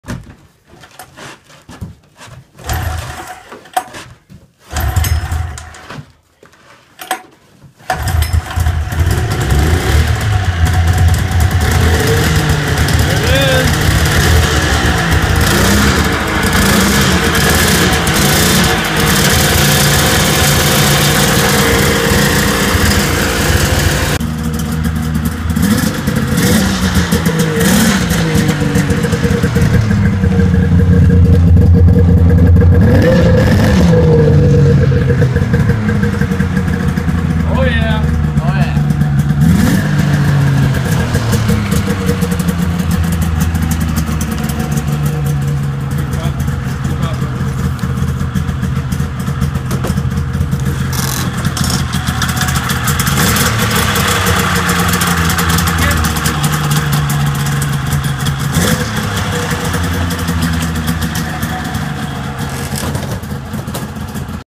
1994 Polaris Indy XLT 600 start up, walk around
Starting up the latest acquisition after a thorough carb cleaning and new gas.